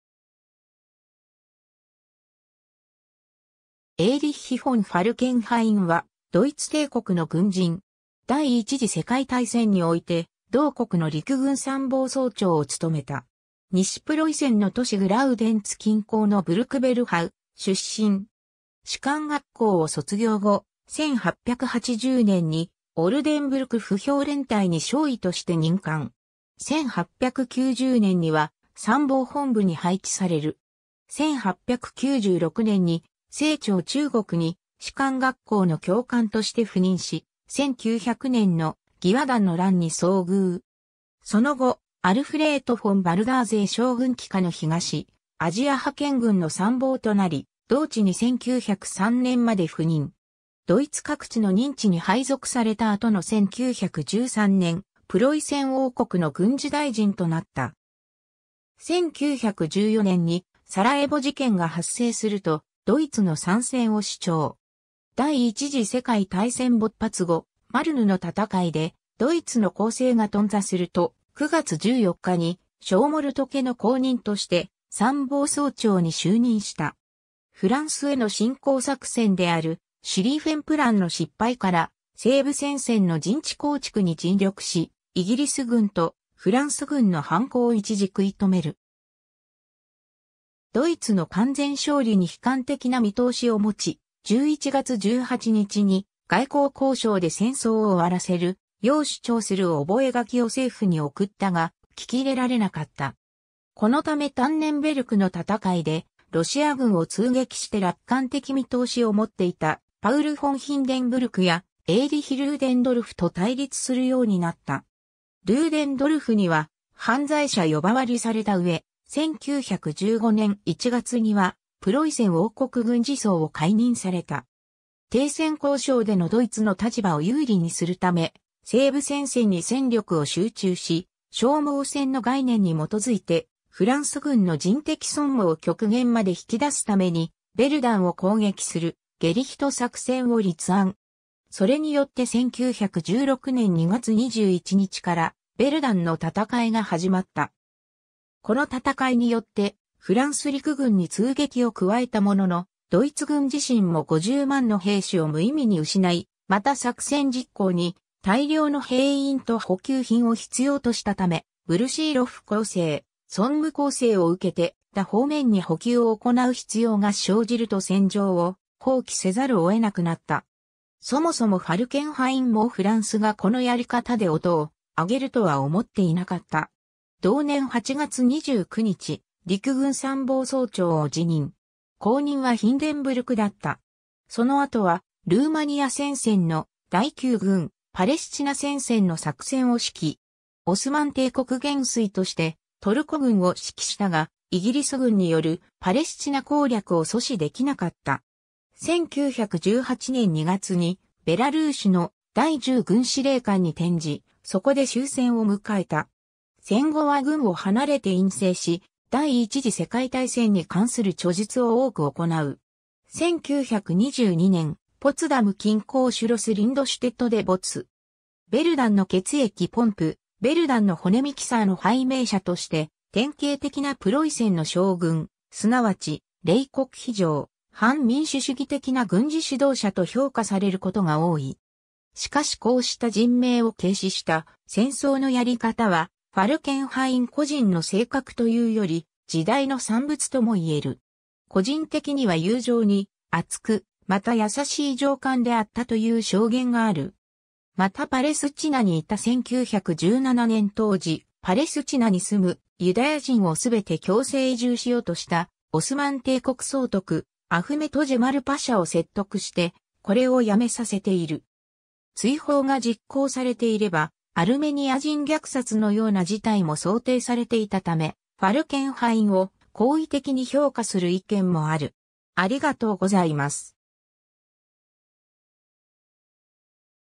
エーリッヒ・フォン・ファルケンハイン
プール・ル・メリット勲章戦功章受章者
エーリッヒ・フォン・ファルケンハイン（Erich von Falkenhayn, 1861年11月11日 - 1922年4月8日）は、ドイツ帝国の軍人。
第一次世界大戦において同国の陸軍参謀総長を務めた。
 西プロイセンの都市グラウデンツ近郊のブルクベルハウ（現在のポーランド・クヤヴィ＝ポモージェ県グルジョンツ郡のビャウォホヴォ）出身。
士官学校を卒業後、1880年にオルデンブルク歩兵連隊に少尉として任官。
1890年には参謀本部に配置される。
1896年に清朝中国に士官学校の教官として赴任し、1900年の義和団の乱に遭遇。
その後、アルフレート・フォン・ヴァルダーゼー将軍麾下の東アジア派遣軍の参謀となり、同地に1903年まで赴任。
ドイツ各地の任地に配属された後の1913年、プロイセン王国の軍事大臣となった。
 1914年にサラエヴォ事件が発生するとドイツの参戦を主張。
第一次世界大戦勃発後、マルヌの戦いでドイツの攻勢が頓挫すると、9月14日に小モルトケの後任として参謀総長に就任した。
フランスへの侵攻作戦であるシュリーフェン・プランの失敗から、西部戦線の陣地構築に尽力し、イギリス軍とフランス軍の反攻を一時食い止める。
ドイツの完全勝利に悲観的な見通しを持ち、11月18日に外交交渉で戦争を終わらせるよう主張する覚書を政府に送ったが、聞き入れられなかった。
このためタンネンベルクの戦いでロシア軍を痛撃して楽観的見通しを持っていたパウル・フォン・ヒンデンブルクやエーリヒ・ルーデンドルフと対立するようになった。
ルーデンドルフには「犯罪者」呼ばわりされたうえ、1915年1月にはプロイセン王国軍事相を解任された。
 停戦交渉でのドイツの立場を有利にするため、西部戦線に戦力を集中し、消耗戦の概念に基づいてフランス軍の人的損耗を極限まで引き出すためにヴェルダンを攻撃するゲリヒト（裁判）作戦を立案。
それによって1916年2月21日からヴェルダンの戦いが始まった。
この戦いによってフランス陸軍に痛撃を加えたものの、ドイツ軍自身も50万の兵士を無意味に失い、また作戦実行に大量の兵員と補給品を必要としたため、ブルシーロフ攻勢、ソンム攻勢を受けて他方面に補給を行う必要が生じると戦場を放棄せざるを得なくなった。
そもそもファルケンハインもフランスがこのやり方で音を上げるとは思っていなかった。
同年8月29日、陸軍参謀総長を辞任。
後任はヒンデンブルクだった。
 その後はルーマニア戦線の第9軍、パレスチナ戦線の作戦を指揮。
オスマン帝国元帥としてトルコ軍を指揮したが、イギリス軍によるパレスチナ攻略を阻止できなかった。
1918年2月にベラルーシの第10軍司令官に転じ、そこで終戦を迎えた。
 戦後は軍を離れて隠棲し、第一次世界大戦に関する著述を多く行う。
1922年、ポツダム近郊シュロス・リンドシュテットで没。
 「ヴェルダンの血液ポンプ」「ヴェルダンの骨ミキサー」の拝名者として、典型的なプロイセンの将軍、すなわち冷酷非情、反民主主義的な軍事指導者と評価されることが多い。
しかしこうした人命を軽視した戦争のやり方は、ファルケンハイン個人の性格というより時代の産物ともいえる。
個人的には友情に篤く、また優しい上官であったという証言がある。
 またパレスチナにいた1917年当時、パレスチナに住むユダヤ人を全て強制移住しようとしたオスマン帝国総督アフメト・ジェマル・パシャを説得してこれをやめさせている。
追放が実行されていればアルメニア人虐殺のような事態も想定されていたため、ファルケンハインを好意的に評価する意見もある(Afflerbach著 Falkenhaynなど)。